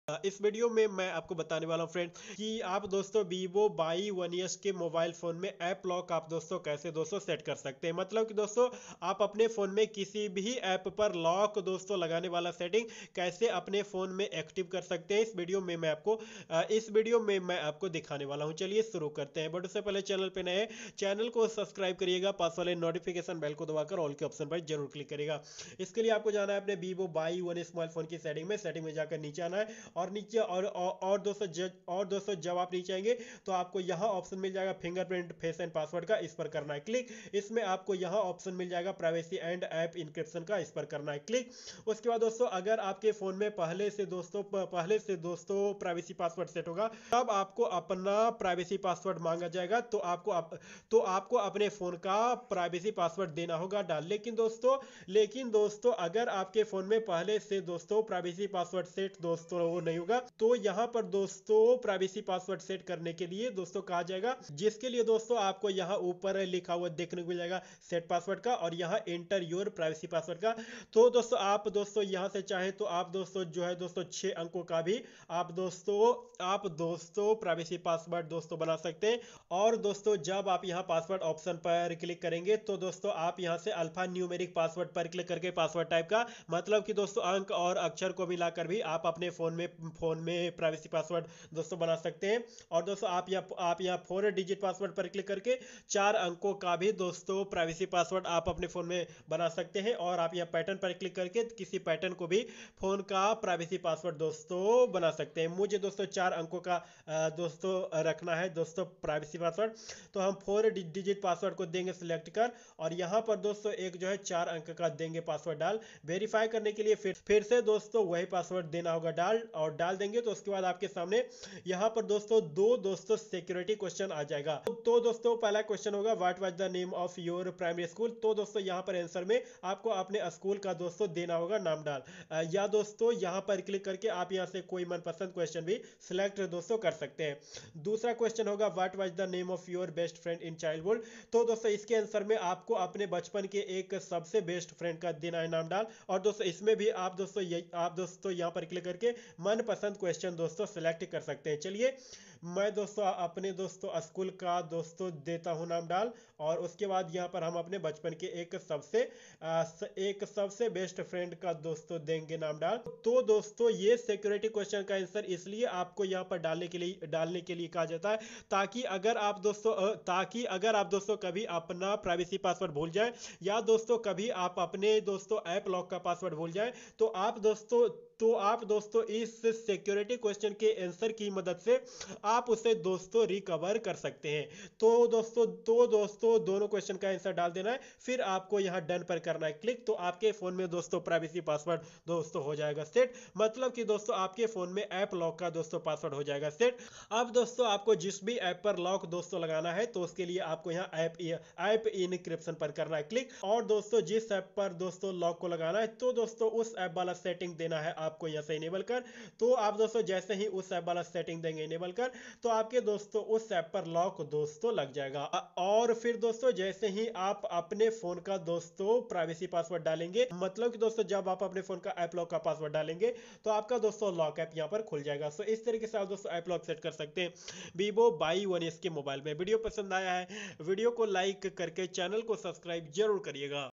Vivo Y1s me app lock set kaise kare।how to set app lock Vivo Y1s।vivo Y1s me app lock kaise lagaye
is video me bataya gaya hai friends ki vivo Y1s me app lock set kaise kiya jata hai